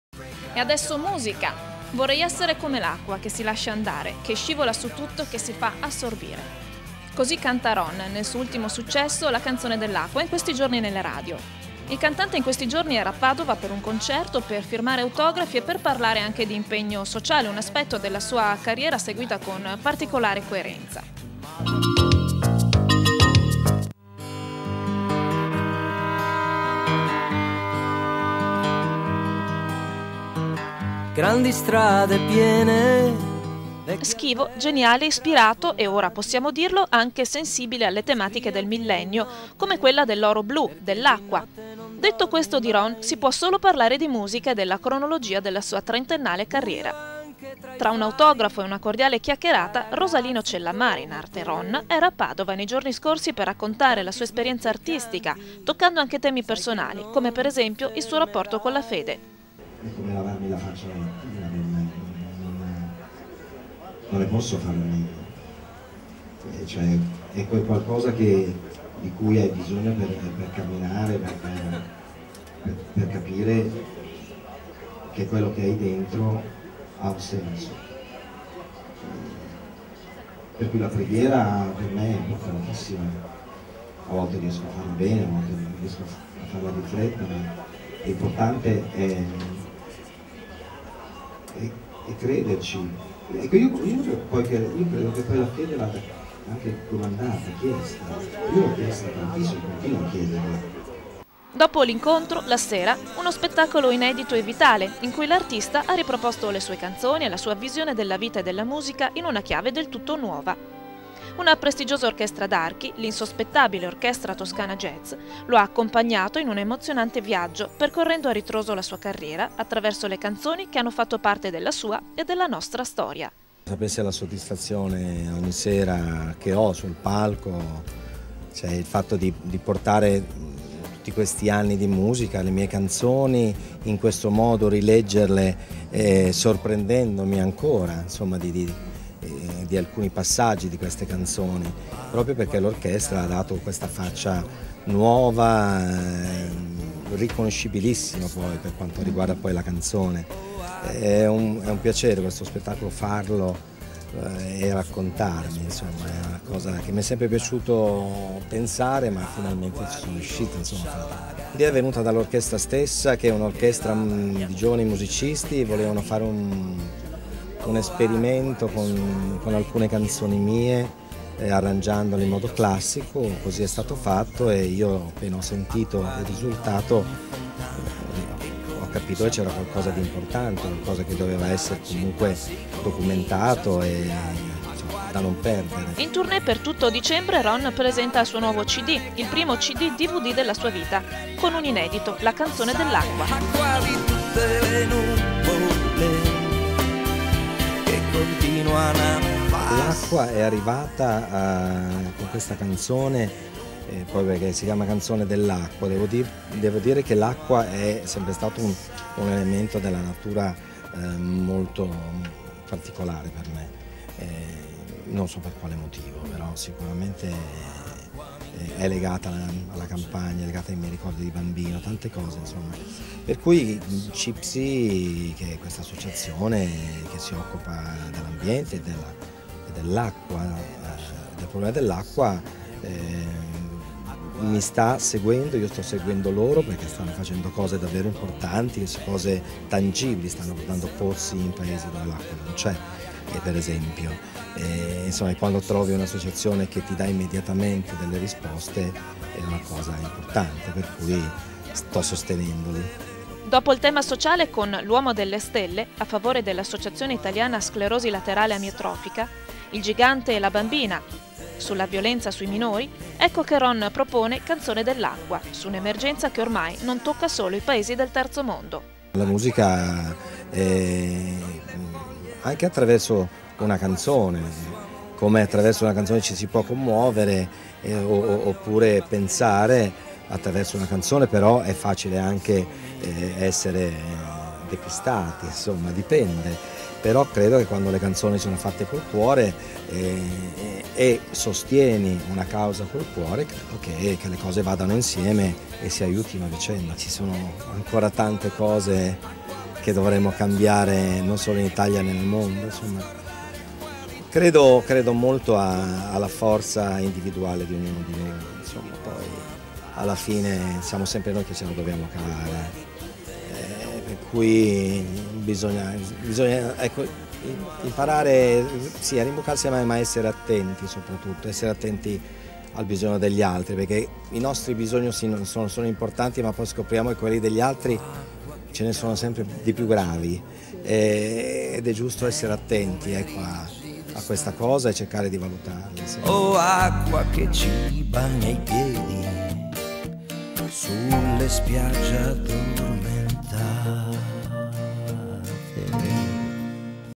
Incontro con Ron
Incontro con il cantante Ron, a Padova per la sua tournèe con l'Orchestra Toscana Jazz